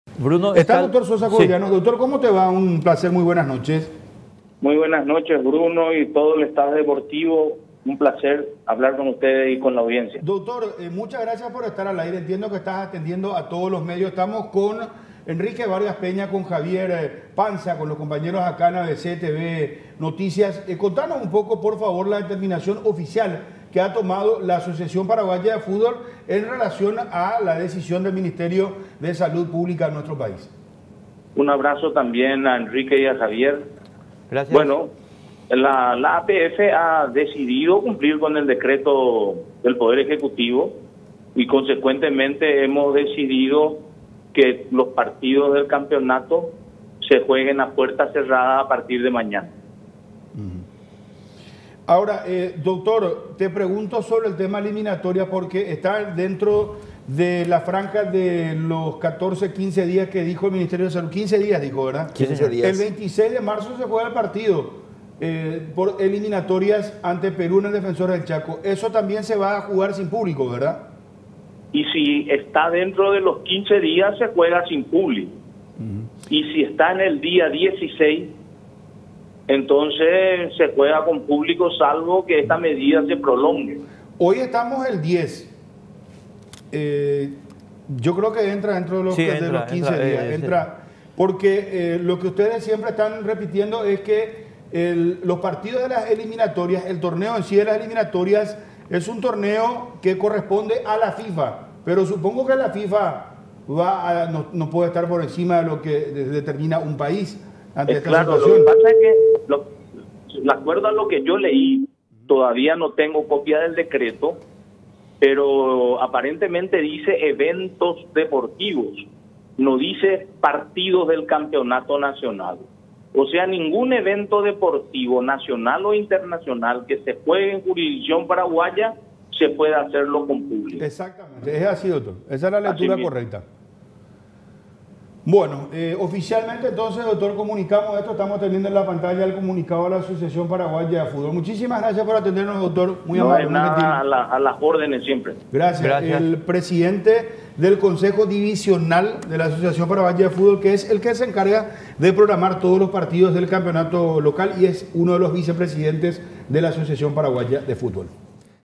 Gobierno suspende actividades: Encuentros deportivos a puertas cerradas
El Gobierno Nacional dispuso la suspensión de todas las actividades que implican aglomeración de personas a fin de evitar la propagación del coronavirus, como afecta esta medida al inicio de las eliminatorias mundialistas y a los demás encuentros deportivos, es lo que responde a ABC Noticias Central en un contacto telefónico el vicepresidente de la APF Carlos Sosa Jovellanos.